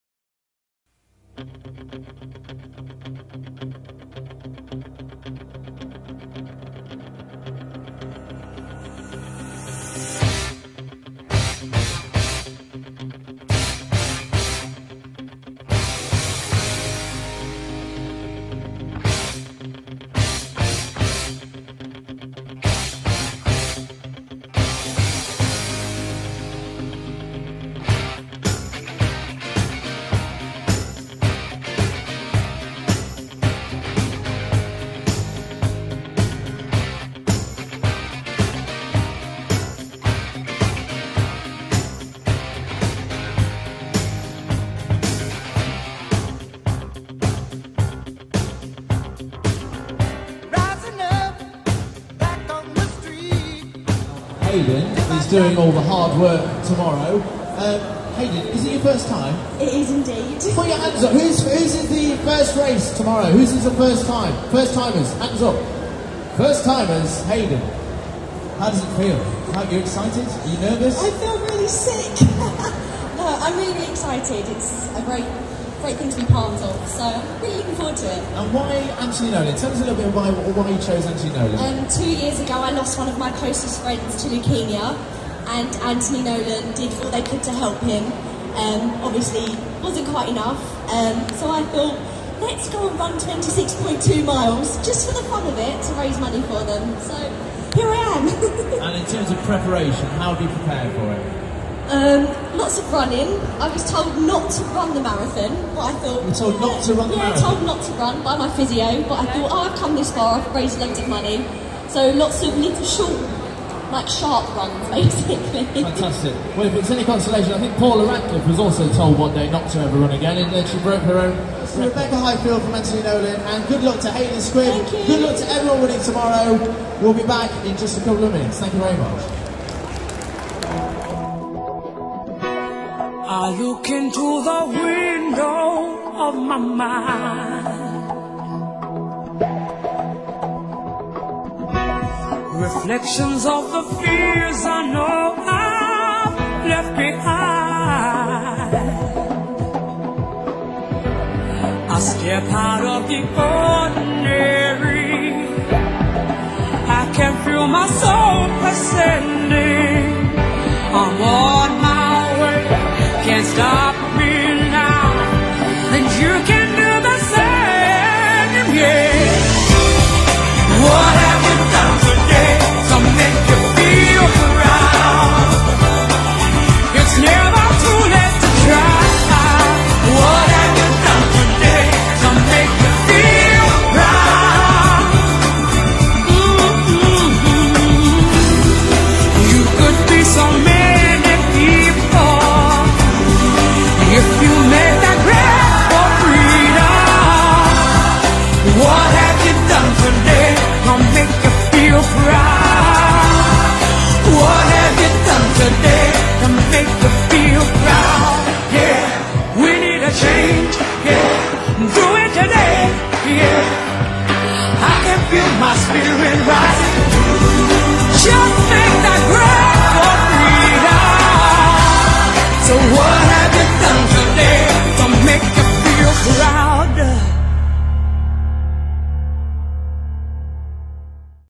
LONDON MARATHON 2015 || ANTHONY NOLAN || VIRGIN MONEY || SQUIBB VICIOUS
On April 26th 2015 I run the London Marathon for Anthony Nolan, here's a short video I made of that weekend.